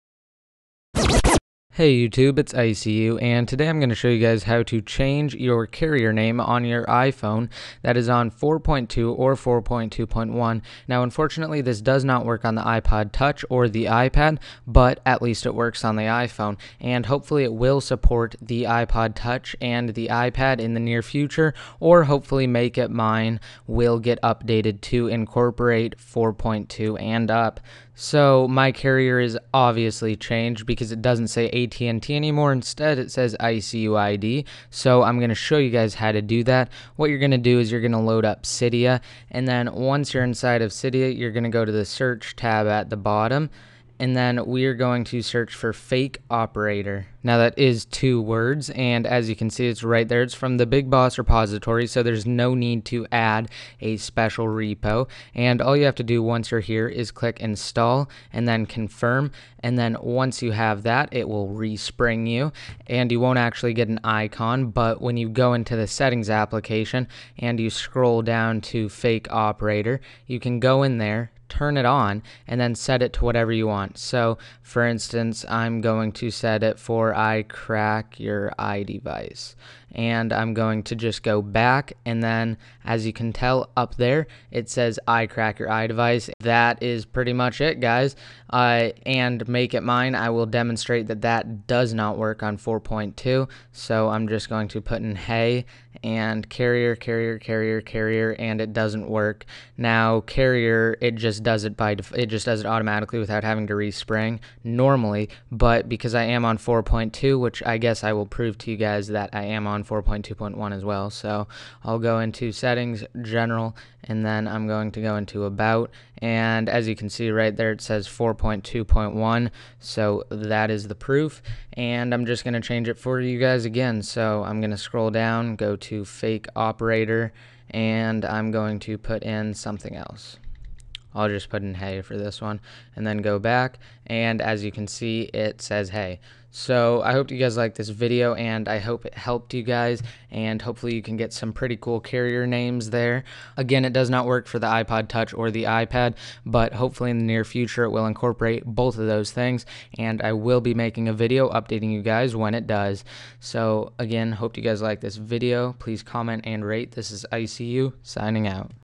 Change Your iPhone's Carrier Name On iOS 4.3.3
Makeitmine (MIM) didn't work for your iDevice on 4.2.1 or 4.2? Well this will and it's free! Unfortunately it doesn't work for the iPod Touch or iPad at this moment tho!

Steps:

1. Open Cydia

2. Search for Fake Operator

3. Install it

4. Go into your settings app and find the Fake Operation section

5. Enable it and set your new carrier name

6. Go back and YOU'VE GOT IT!